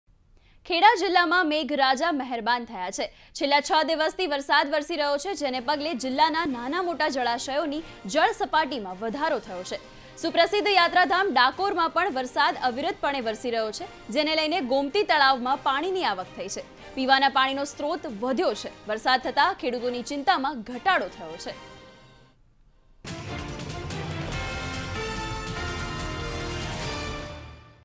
યાત્રાધામ ડાકોરમાં કમરસમા પાણી ગળતેશ્વર તળાવમાં નવા નીરની આવક
"New water inflow in Galteshwar Lake Dakor"

G9 Gujarati, the most trusted broadcast, delivers real unedited coverage and conversations to audience.